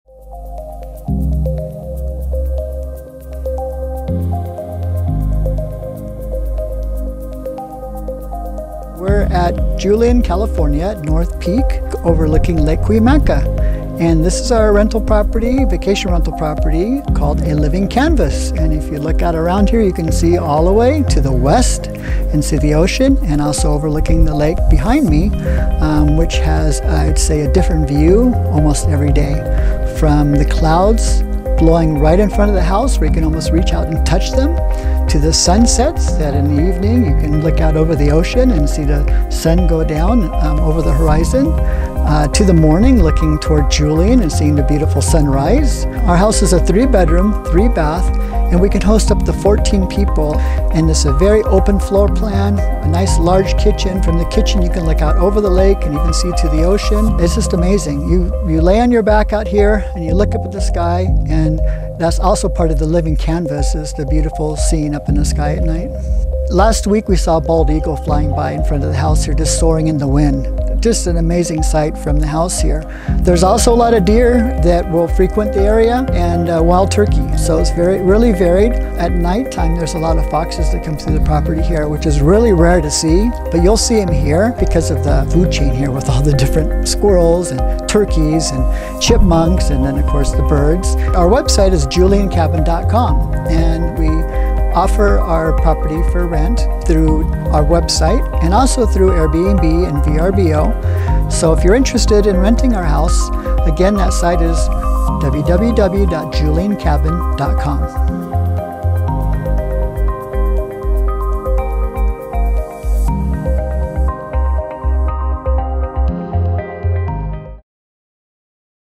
A Living Canvas | Lake Cuyamaca Cabin
Check out one of Julian’s most beautiful vacation rental properties on North Peak, overlooking Lake Cuyamaca. This one of a kind property is situated at over 5,000 feet elevation and features stunning views to both Lake Cuyamaca and to the ocean towards the west. This is one of the most unique accommodation experiences that Julian has to offer. Inspired by the ever-changing views and beautiful mountain ambiance, the owner John Leslie calls this place “A Living Canvas".

Book your group retreat now on Airbnb and experience San Diego’s Backcountry like never before